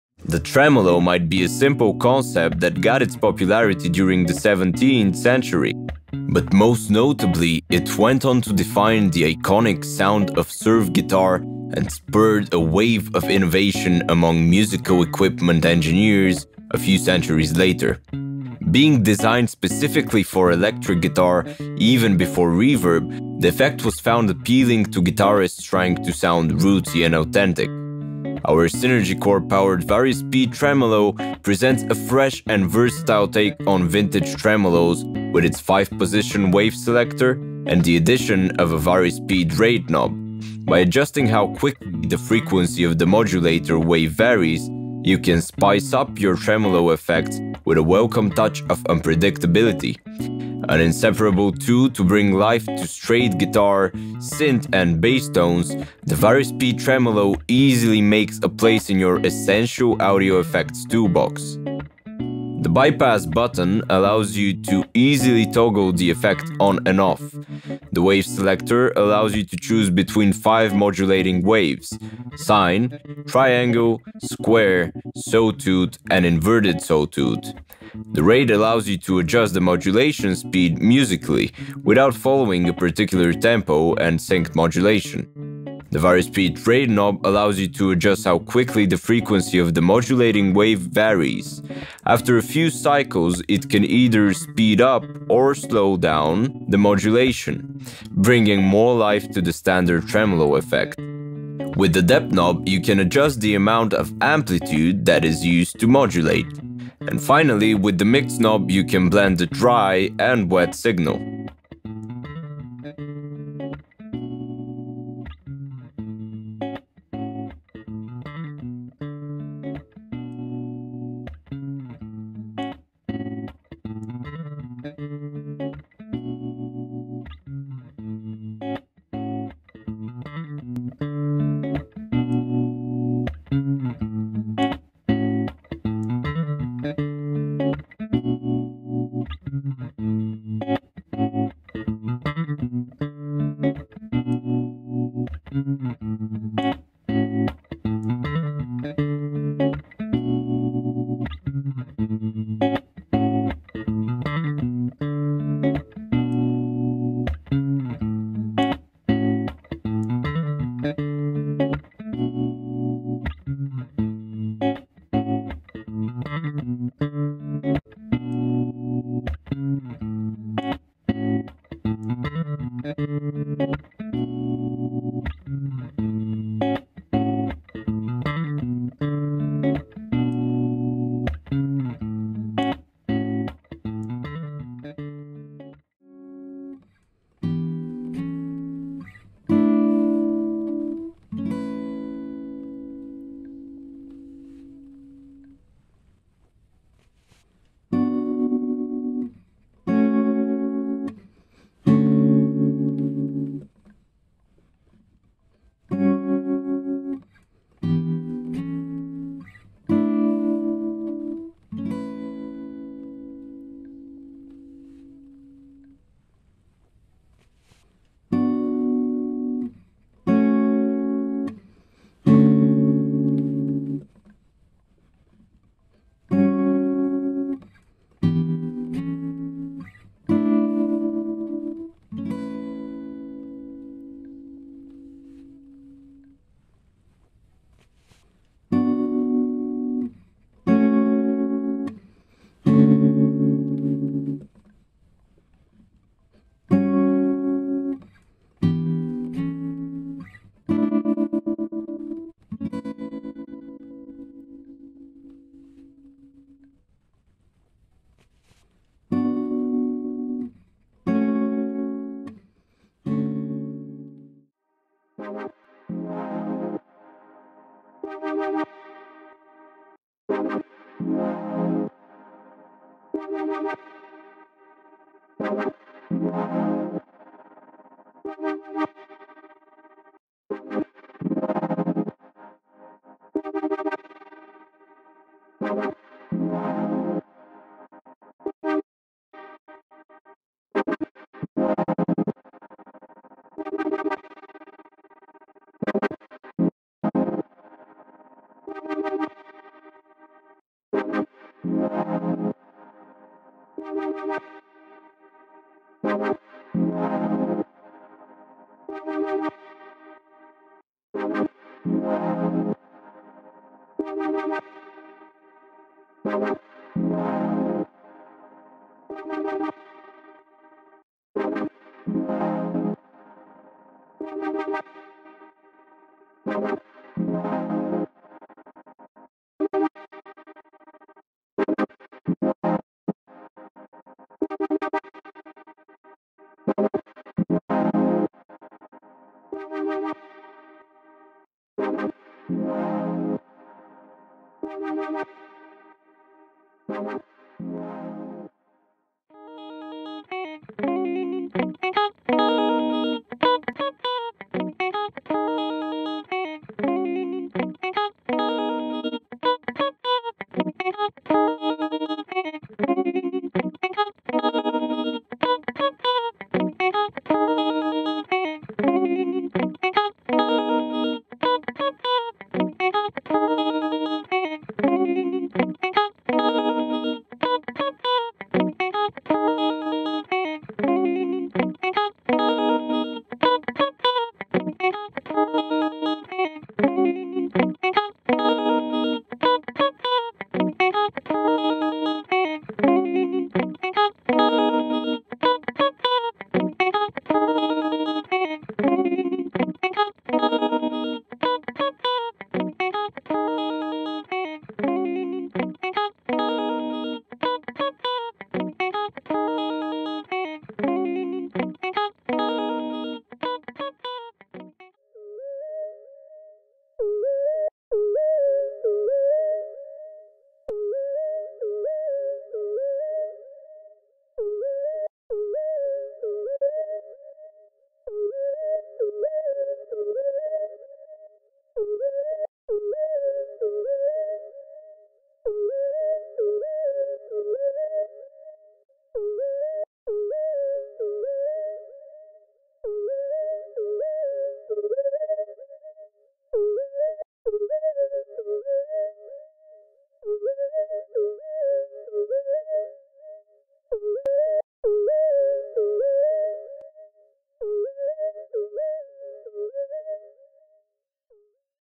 Vari-Speed Tremolo | Guitar Modulation | Real-time Effect
Being designed specifically for electric guitar, even before reverb, the tremolo effect was found appealing to guitarists trying to sound rootsy and authentic. An inseparable tool to bring life to straight guitar, synth, and bass tones, the Vari-Speed Tremolo easily makes a place in your essential audio effects toolbox.

The Vari-Speed Tremolo is available in the expandable library of all Synergy Core interfaces. Monitor with the effect in real-time when recording or use it when mixing or mastering in your DAW without adding any CPU load.

About Antelope Audio
Antelope Audio is a leading manufacturer of analog and digital pro audio equipment such as interfaces, modeling microphones, and converters.